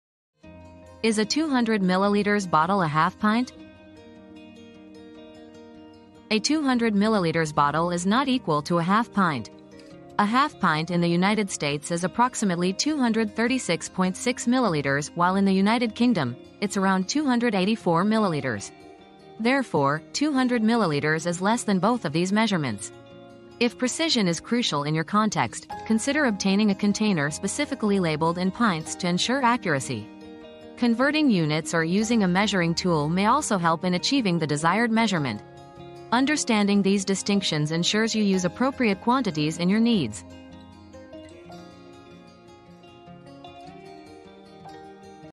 Is a 200ml bottle a half pint?
Is a 200ml Bottle a Half Pint? The Surprising Truth! • 200ml vs Half Pint • Discover the surprising truth about 200ml bottles and half pints! Learn how these measurements compare in the US and UK, and why precision matters in your measurements. Dive into the details to ensure you're pouring the right amount every time!